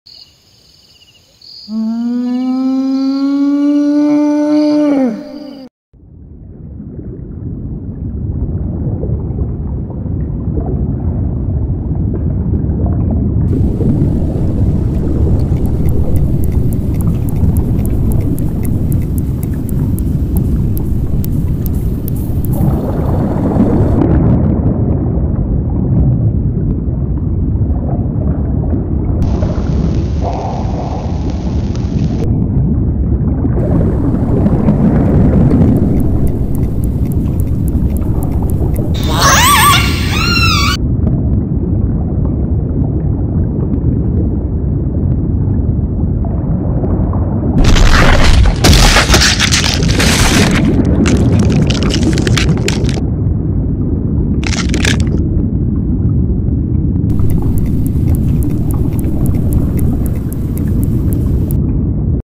Killer whale vs Great white shark / Claymotion sea animals fight
Мы снова опускаемся в пучину океана и вы можете видеть битву двух королей современной водной стихии. Это косатка - самый огромный дельфин и большая белая акула, тот самый акула - людоед, которого мы знаем. Это будет воистину жарче и  я надеюсь вам понравится.